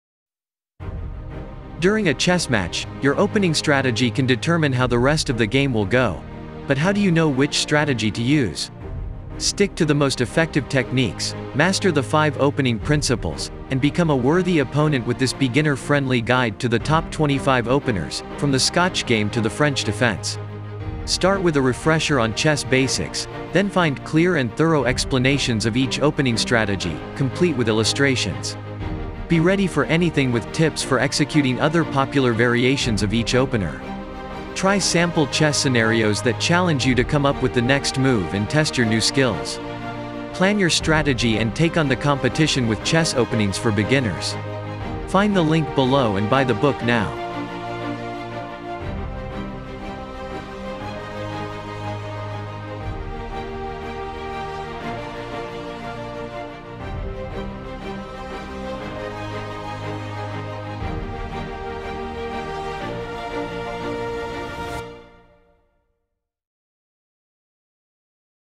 Chess Openings for Beginners
Chess Openings for Beginners: Essential Strategies Every Player Should Know

Make your first move with confidence

During a chess match, your opening strategy can determine how the rest of the game will go—but how do you know which strategy to use? Stick to the most effective techniques, master the five opening principles, and become a worthy opponent with this beginner-friendly guide to the top 25 openers, from the Scotch Game to the French Defense.

What sets this book apart from other chess strategy books:

Step-by-step advice—Start with a refresher on chess basics, then find clear and thorough explanations of each opening strategy, complete with illustrations.

The most common variants—Be ready for anything with tips for executing other popular variations of each opener.

Ways to practice what you've learned—Try sample chess scenarios that challenge you to come up with the next move and test your new skills.

Plan your strategy and take on the competition with Chess Openings for Beginners.